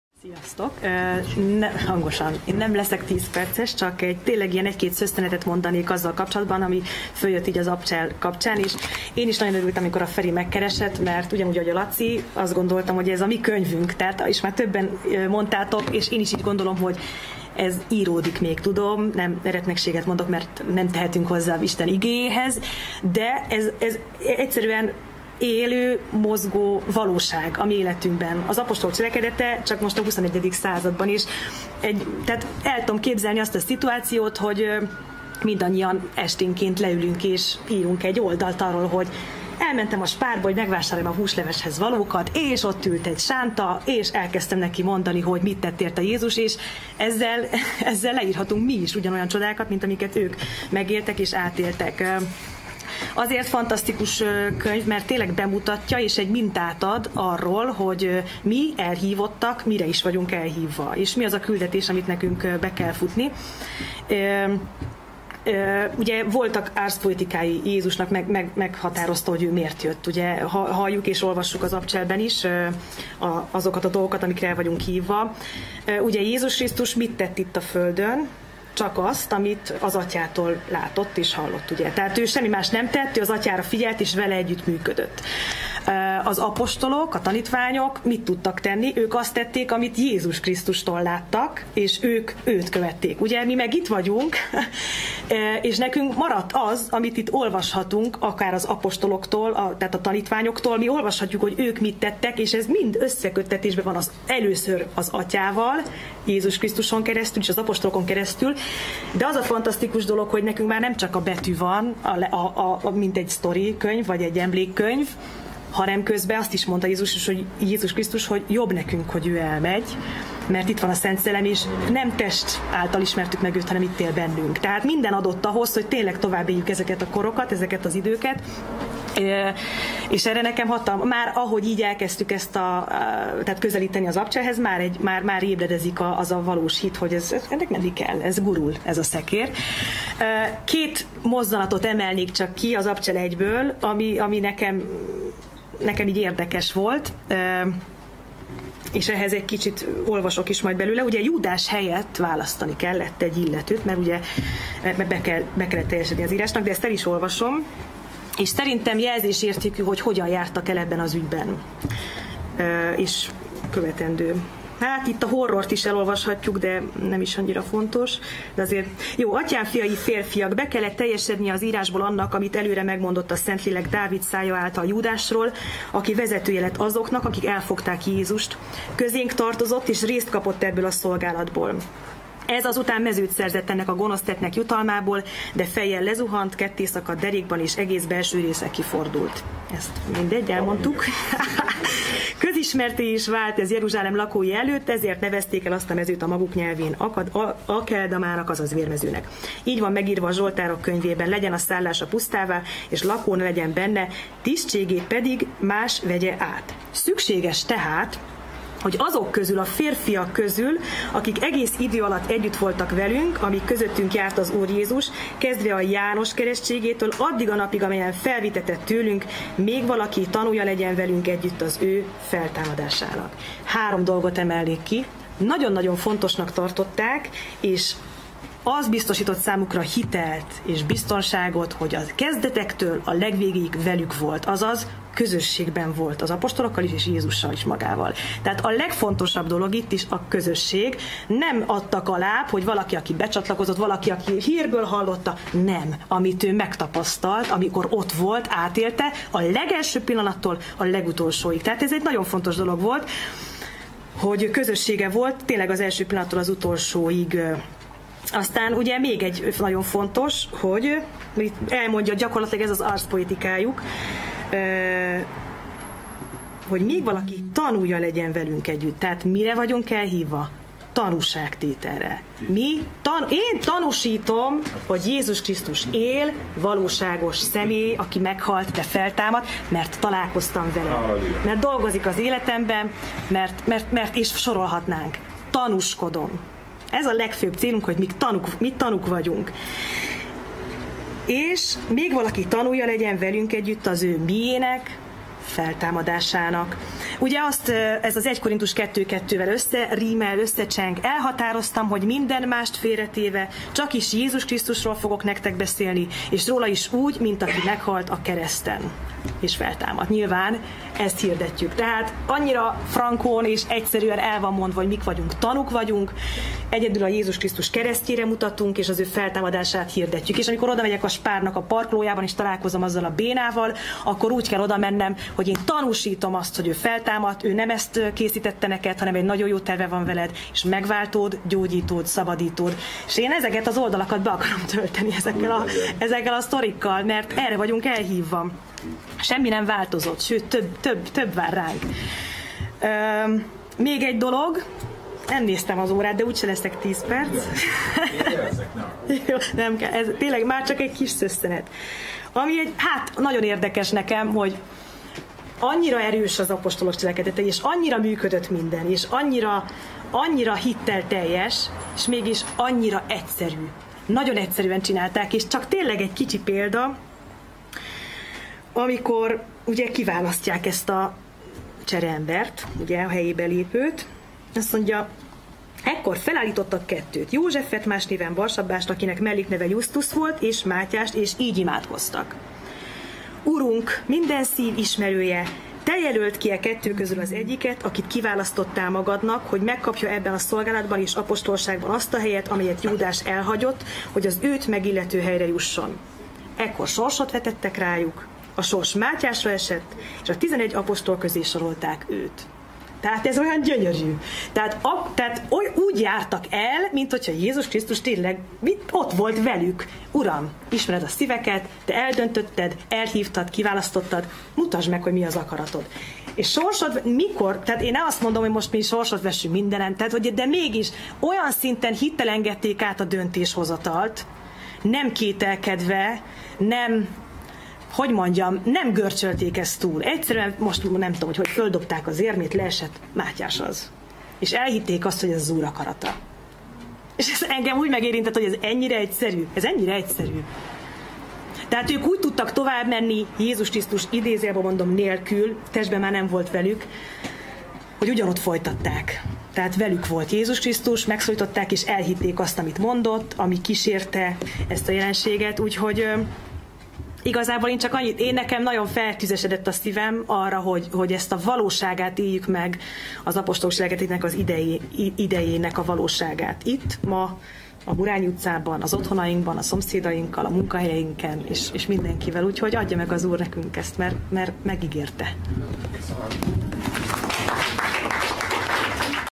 Apostolok cselekedetei I.- Fóris Rita
Az Apostolok –  a Szent Szellem, a Tanítványok, az Új teremtés – cselekedeteit szeretnénk fejezetről fejezetre, közösen tanulmányozni.
Hogyan vezette Isten azokban az időkben, a tanítványokat?
Hogyan hallották meg, ismerték fel Péter és Pál és a többi tanítványok a Szellem hangját?
Hogyan és milyen módokon nyilvánult meg a tanítványokon keresztül Isten természete, ereje?
Hogyan vezet ma Isten Szelleme?
Hogyan és miket cselekszi ma, köztünk, általunk